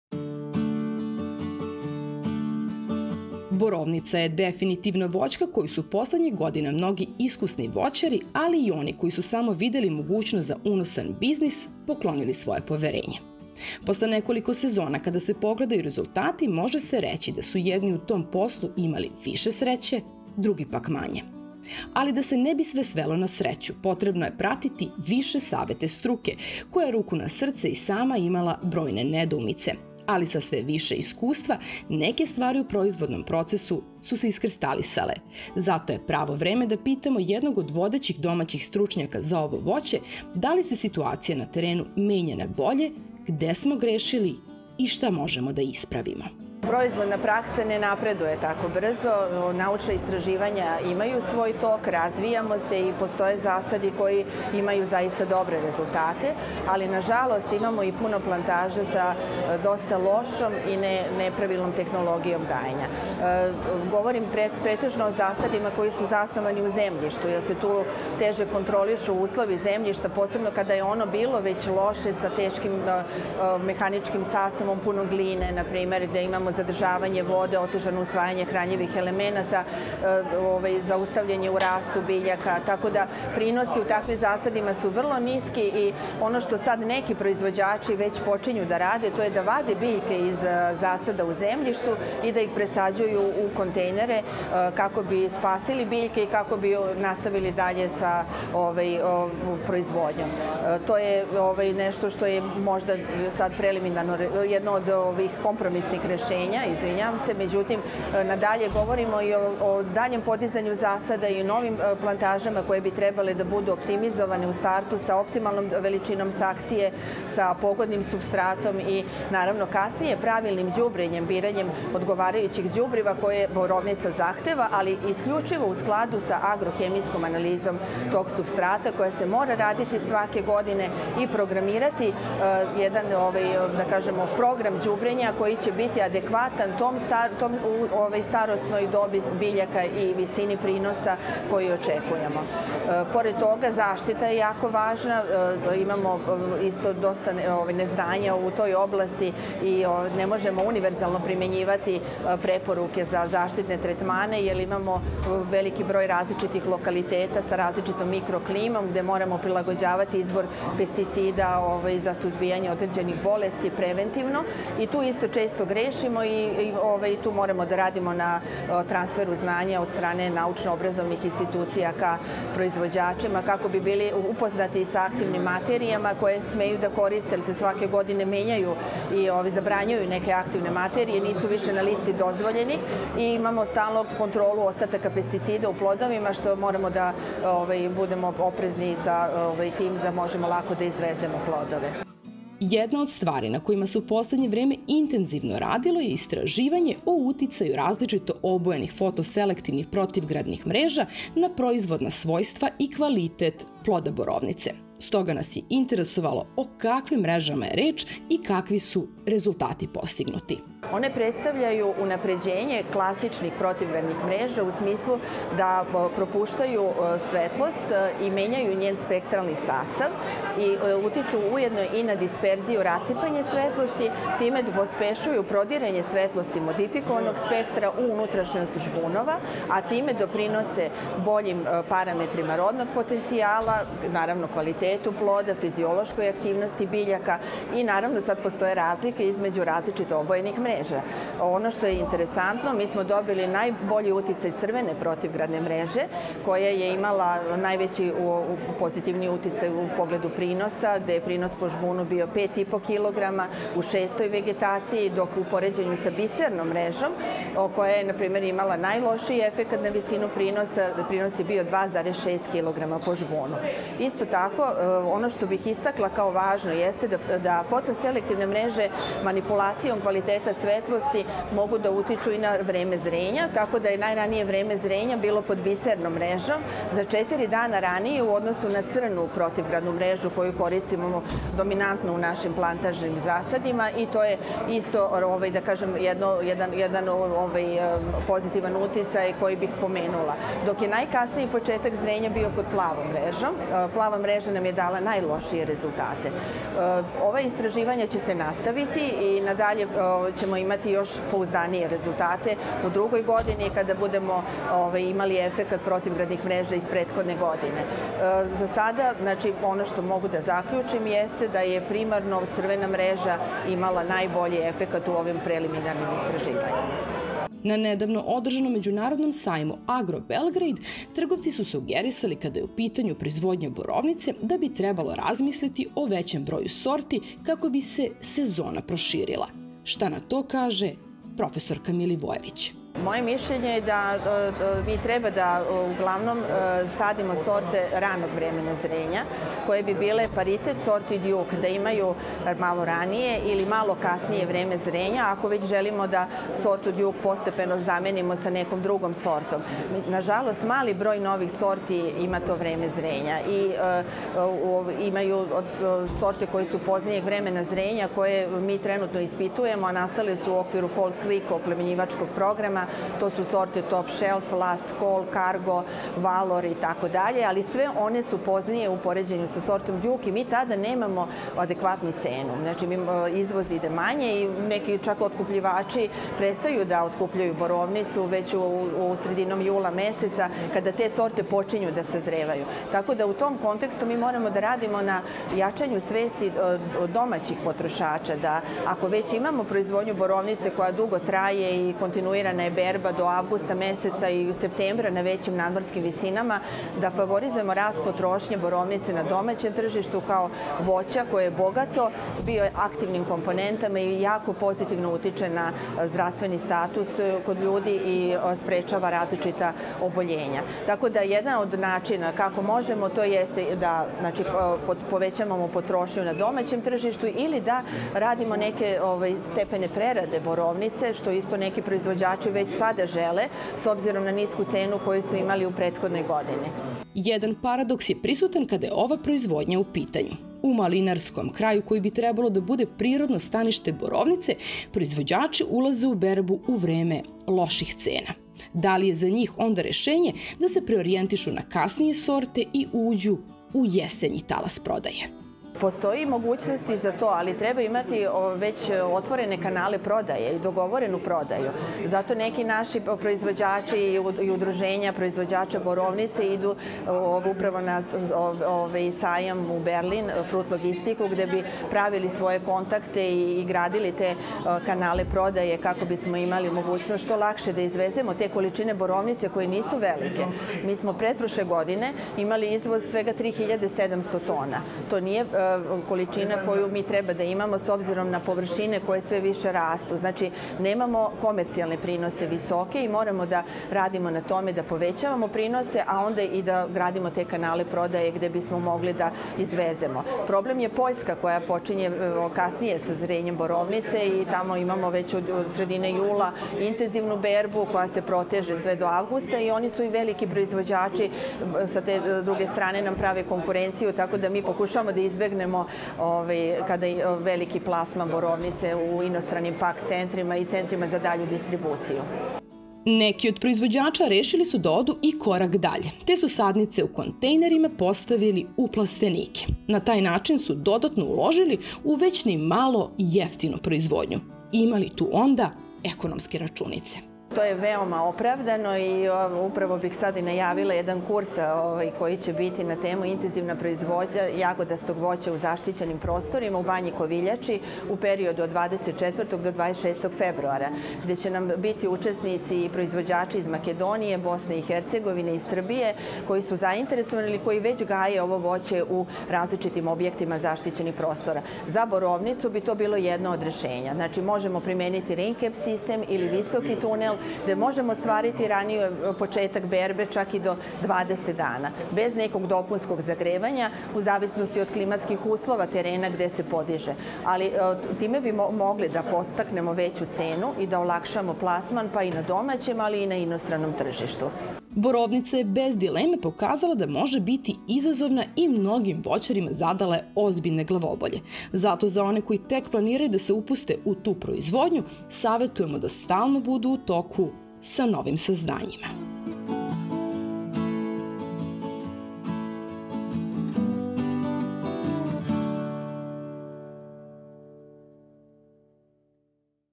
Kako do VIŠE NOVCA u proizvodnji BOROVNICE?
Kojim tehnološkim inovacijama se plantaže borovnice mogu podići na viši nivo?
Sagovornica: Prof. dr Jasminka Milivojević – Poljoprivredni fakultet u Beogradu
Facebook: Farma emisija o poljoprivredi